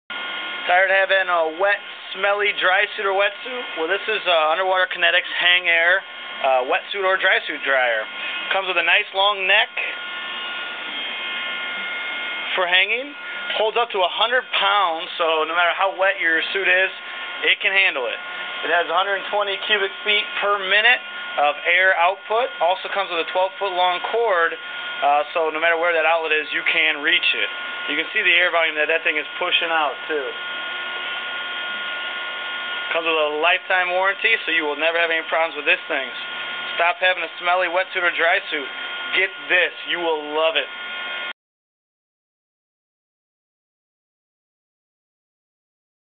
UK Hangair Hanger
Underwater Kinetics Hangair Hanger for drying your wetsuit or drysuit.